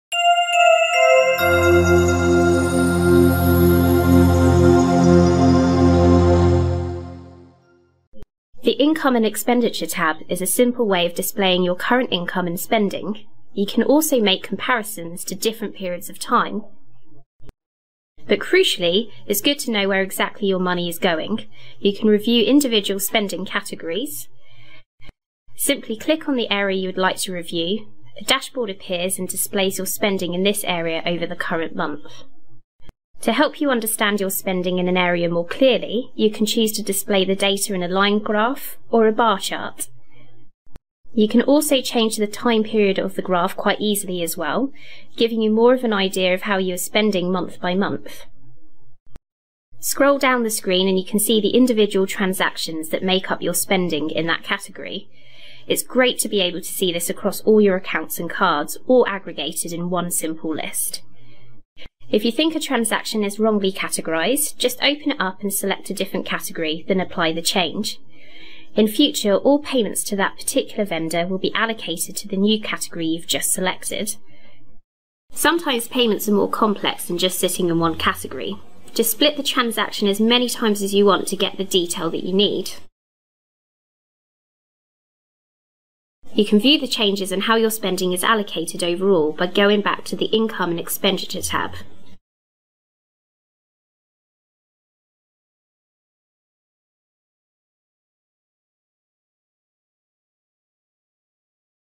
Spending areas
Adding detail to your spending area on the Personal Finance Portal